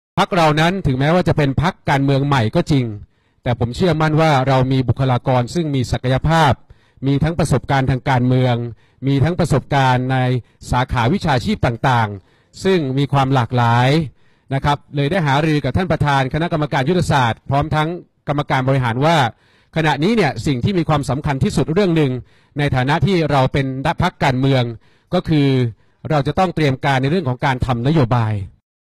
ถึงเรา จะเป็นพรรคใหม่​ แต่เชื่อมั่นว่า มีบุคลากร ที่มีคุณภาพ ปสก. !! " ปรีชาพล พงษ์พานิช " ท ษ ช
" ถึงเราจะเป็น พรรคการเมืองใหม่​ แต่ผม เชื่อมั่นว่า เรามีบุคลากร ที่มีคุณภาพที่มี ทั้งประสบการณ์ทางการเมือง  ประสบการณ์ในสาขาวิชาชีพต่าง ๆ  ซึ่งมีความหลากหลาย ขณะนี้สิ่งสำคัญที่สุดเรื่องหนึ่ง ในฐานะที่เราเป็นพรรคการเมือง ก็คือ เราต้องเตรียมการในเรื่องการทำนโยบาย "

ร.ท.ปรีชาพล พงษ์พานิช
หัวหน้าพรรคไทยรักษาชาติ